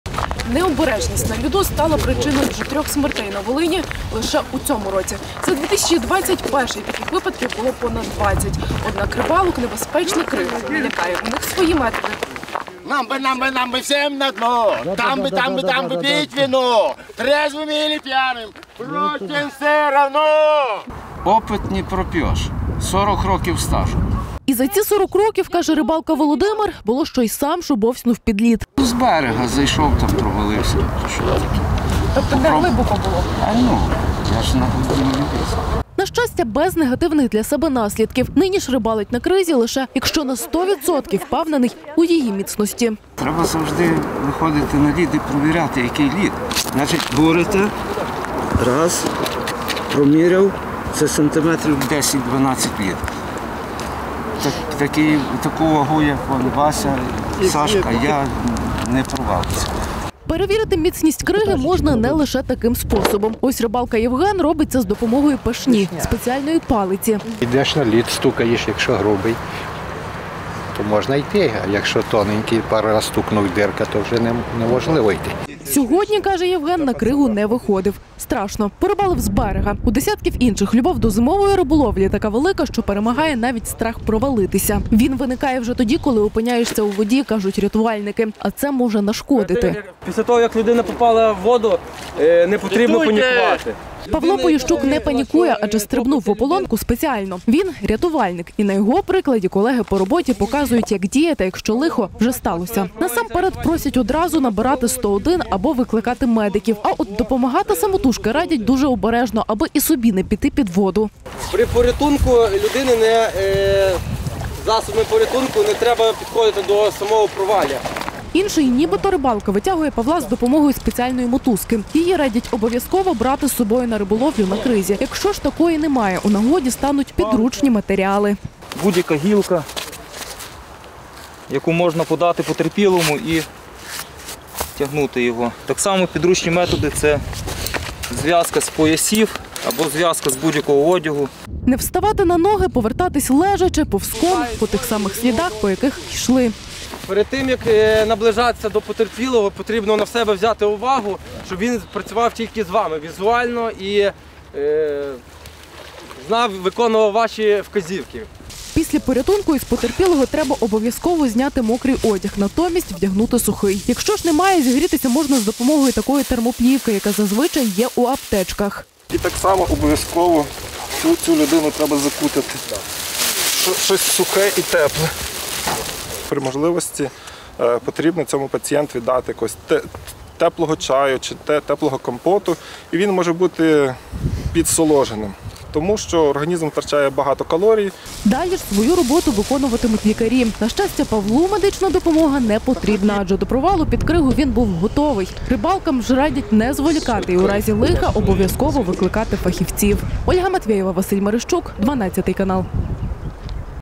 Провалився під лід: що робити?
Годинами із вудкою біля ополонки. Рибовлю зимою на кризі особливо люблять рибалки. В таку погоду на Стиру їх десятки. Небезпечний та підступний лід, якщо їх і лякає, то бажання порибалити - переважає. Нашій Ользі Матвєєвій рибалки розповіли, як впевнитися, що заняття не завершиться лихом, а рятувальники - як діяти, якщо воно вже сталося.